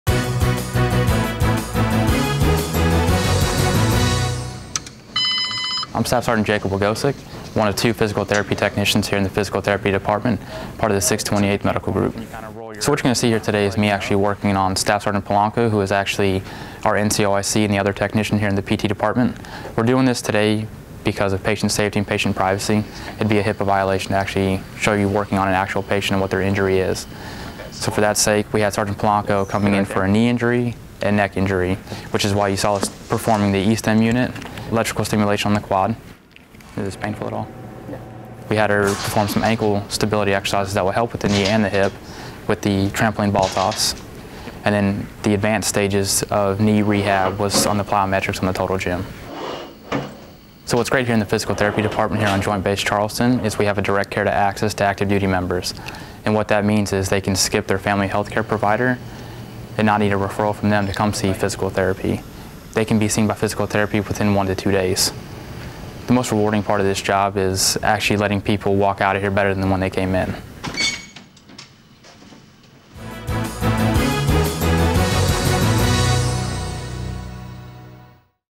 Improving Lives Through Physical Therapy
Joint Base Charleston physical therapists walk us through some of things they offer the Lowcountry warriors to help improve their movement, pain and lives. Produced by A1C Kahdija Slaughter and SrA Jason Treffry.